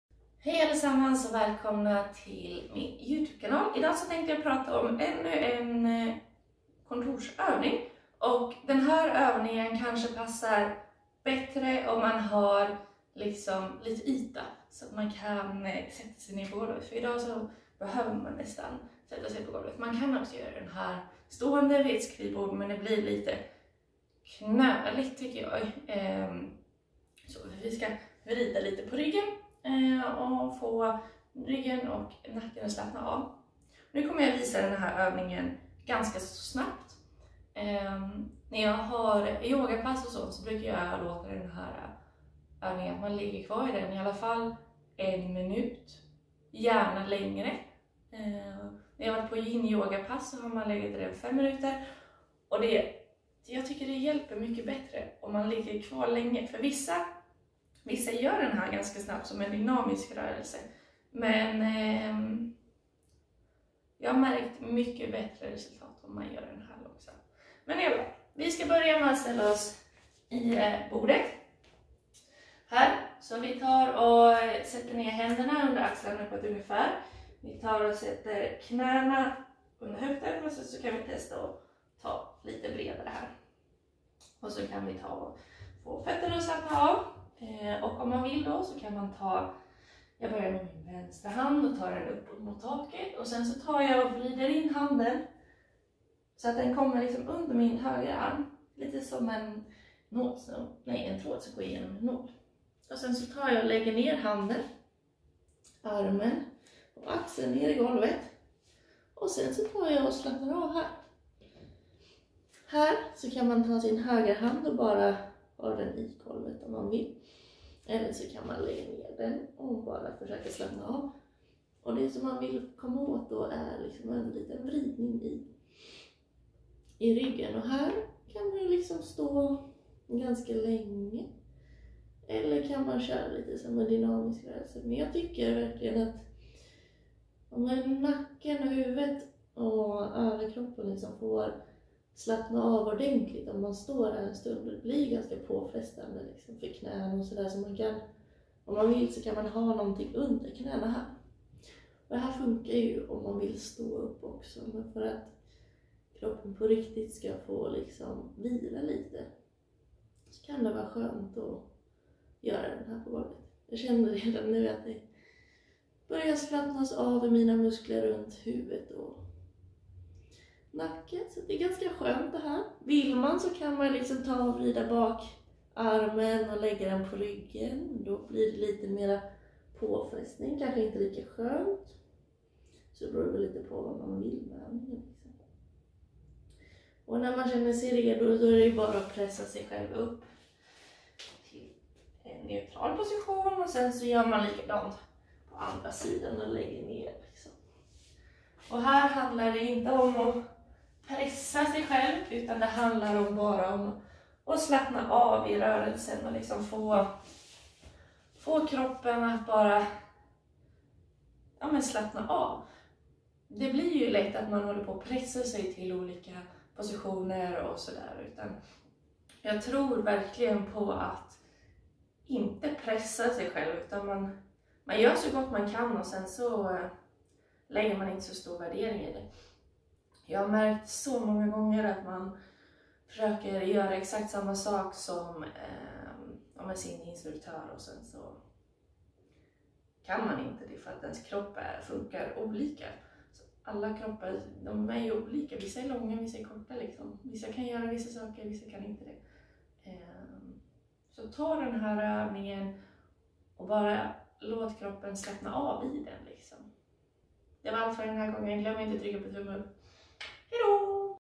Avslappnande vridning för ryggen
Hej! Här kommer en ny övning till min serie med kontorsgympaövningar!

Vill du anmäla dig till serien och få alla avsnitt direkt i din mail?

Den här rörelsen kan man köra antingen dynamiskt eller statiskt. Jag gillar främst att köra den statiskt och försöka bara slappna av i överkroppen i flera minuter.

Personlig tränare och influencer på Clara Färdiga Gå